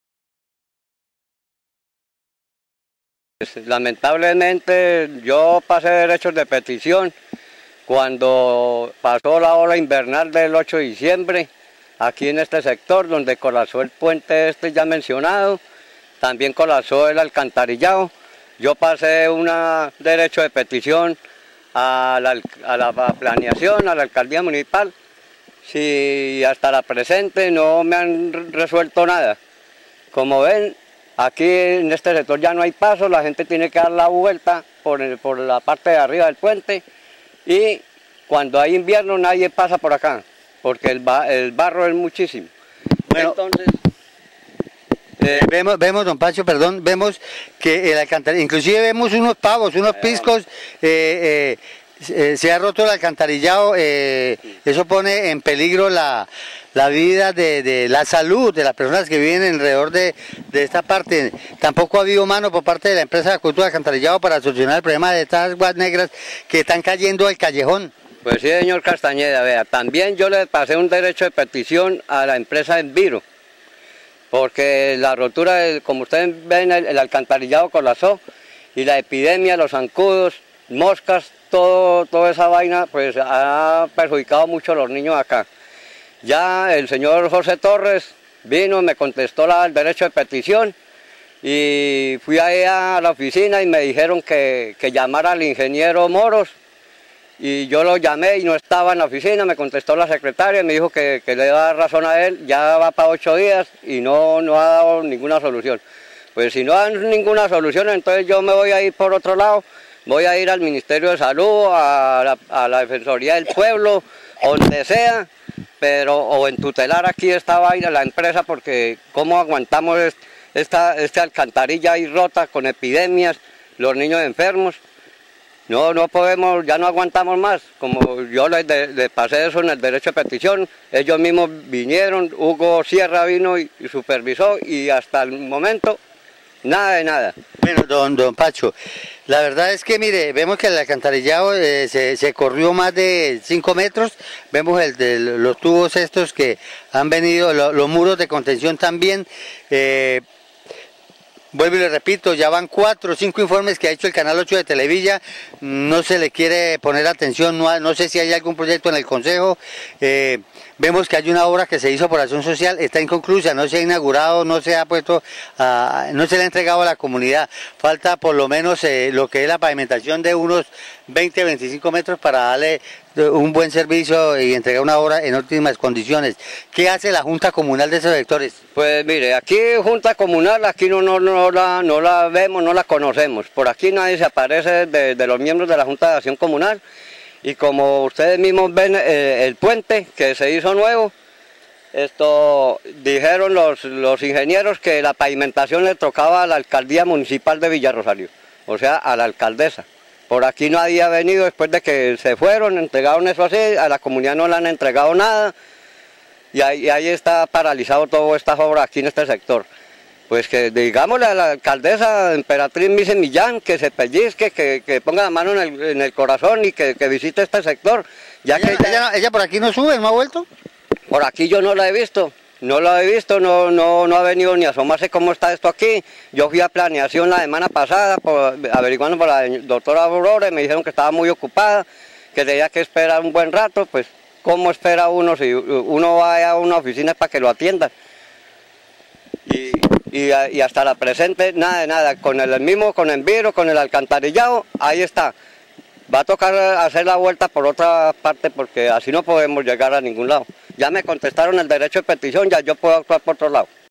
Habitantes del Barrio La Palmita solicitan reparación de puente y arreglo de alcantarillado
Televilla Canal 8.  Habitantes del Barrio La Palmita solicitan reparación de puente y arreglo de alcantarillado